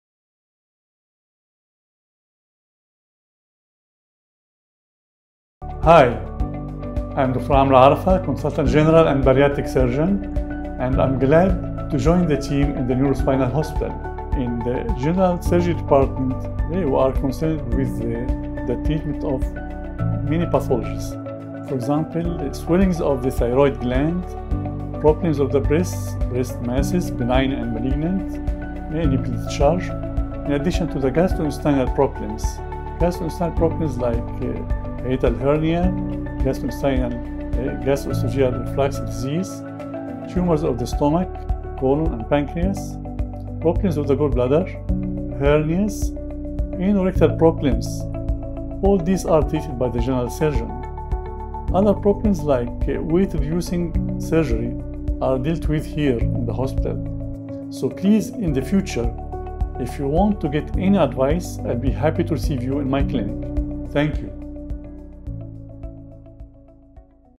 What are the Types of General Surgery Procedures? | Health Talk by Dr. Amr Arafa, General Surgeon
Meet Dr. Amr Arafa, Consultant General Surgeon at NSH, Dubai. With over 38 years of experience, Dr. Amr is skilled in Bariatric Surgery, General Surgery, Laparoscopy, Emergency Surgery, Healthcare Information Technology, and Health Care Services.